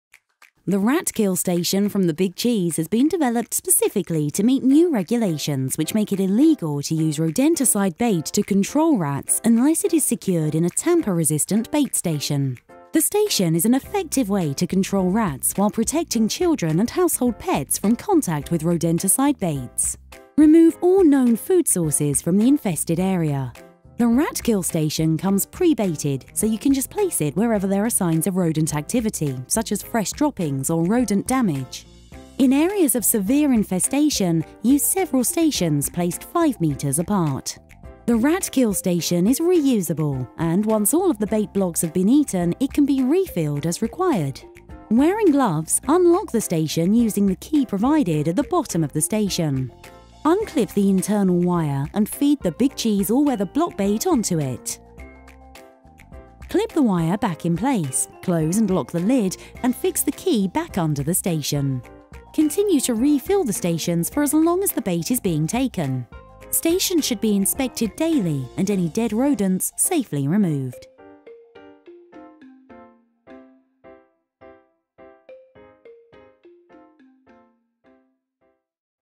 Rat Kill Station (STV133) from The Big Cheese
Compact and ready baited, the Rat Kill Station helps protect children and pets from contact with bait when controlling rats.